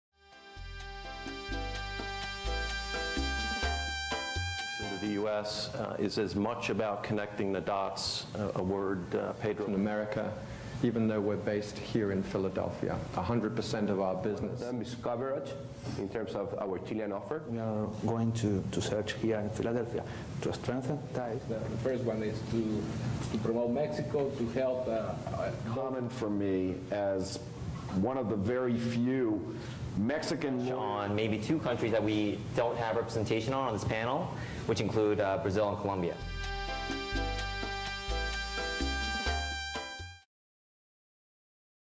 Latin America 2014: Economic, Business, and Trade Forecast
Chilean Chamber of Commerce of Greater Philadelphia Latin America 2014 Breakfast Seminar Intro